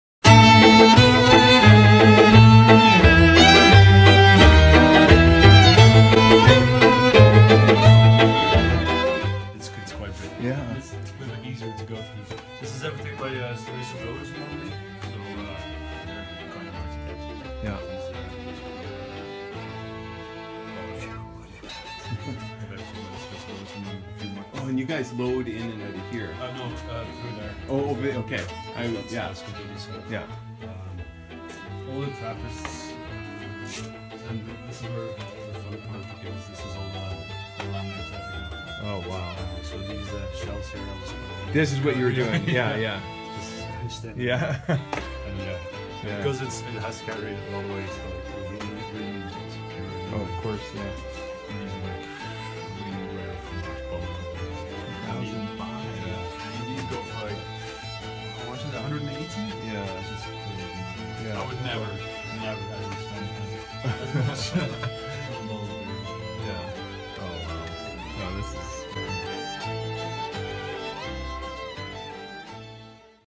Cafe Rose Red Beer Cellar
Gilles gives us a tour of the amazing beer cellar under the Café Rose Red in Bruges, Belgium.